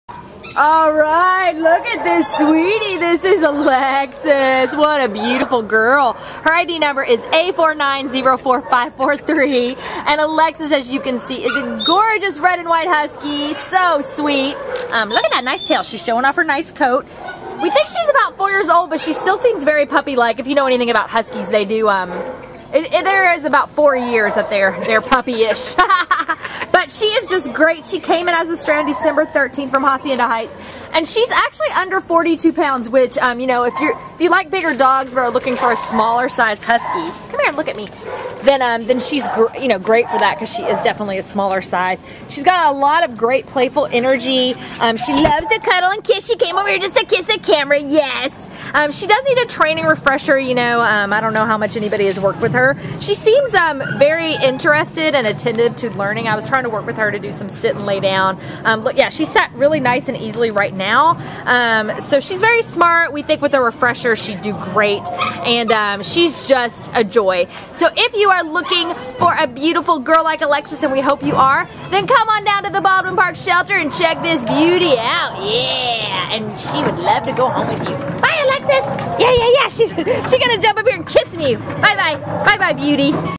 A4904543 Alexis | Siberian Husky
A4904543 Alexis is an exquisite 4-year-old red-and-white female Siberian Husky mix who was found in Hacienda Heights and brought to the Baldwin Park Animal Care Center on December 13th. Weighing 41 lbs, Alexis has a lovely playful energy; she enjoys cuddling and giving kisses to her handler! She can benefit from some basic training refreshers, including leash walking. Since she is eager to please her handler, training should be a breeze. Alexis gets along very well with other dogs, and is even quite gentle with ones smaller than her. Graceful and mild mannered, this happy girl will make an awesome indoor companion for a family or single person living in a private home with a well-fenced yard.